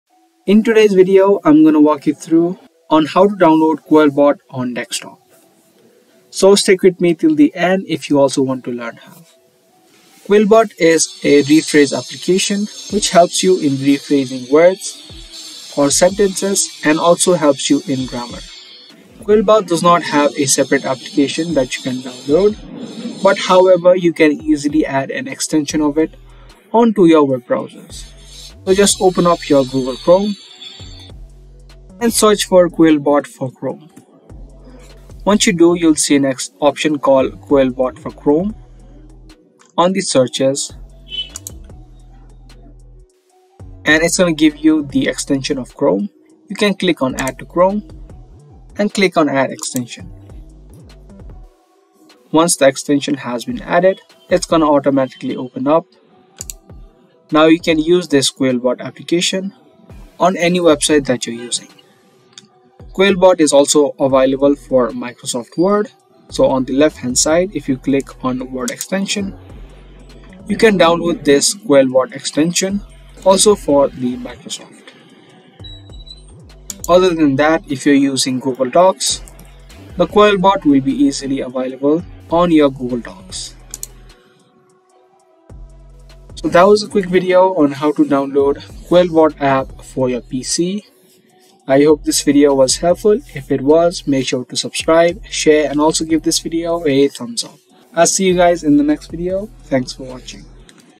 How To Download/Install QuillBot On Desktop (2023)
In this video I'll be showing you how to download and install quillbot app on your desktop /pc / laptop. So watch the video till the end so that you won't miss any steps on how to install quillbot app in windows.